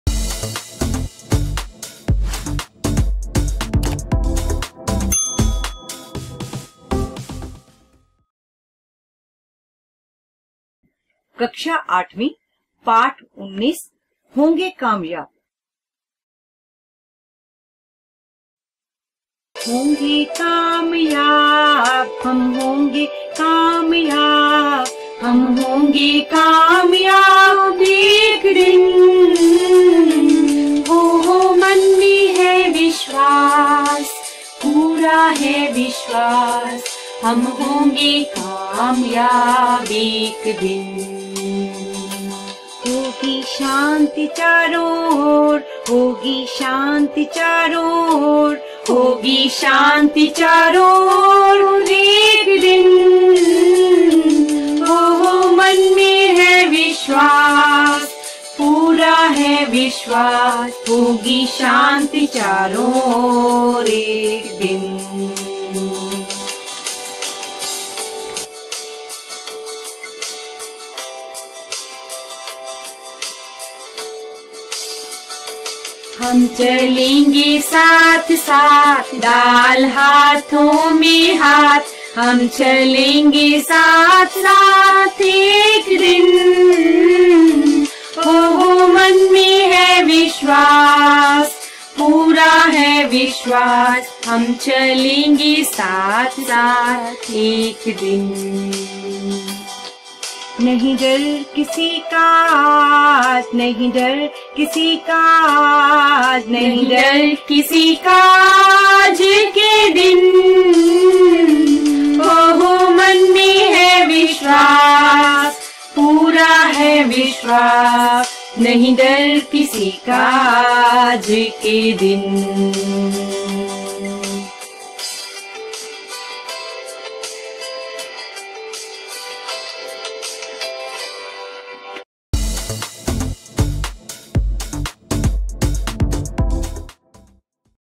PB | Class 8 | Hindi | Chapter 19 | Hoge Kamyaba (PSEB)
अध्याय 19 'होगे कामयाब' पंजाब बोर्ड की कक्षा 8 हिंदी के पाठ है। इस पाठ में हम सुन्दर लाल बहुगुणा द्वारा लिखित एक छोटी सी कहानी सुनते हैं जिसमें एक बच्चे के सपनों और उसकी उन सपनों को पूरा करने की इच्छा का वर्णन किया गया है।

इस पाठ में हम एक गरीब परिवार से संबंधित हैं जो अपने बच्चों के भविष्य के लिए सब कुछ संभव करना चाहता है। इस पाठ के माध्यम से हमें समझ मिलती है कि शिक्षा एक ऐसी चीज है जो हमें संघर्ष करने और सफल होने की ऊर्जा प्रदान करती है।

इस कहानी में बच्चे को एक बड़े संगीत संग्रहालय का दौरा करने का मौका मिलता है जहां उसे संगीत से बहुत प्यार होता है। इस दौरे में उसे इंस्ट्रुमेंट पर गाना बजाने का मौका भी मिलता है जो उसकी जिंदगी का सफलता का मार्ग खोलता है।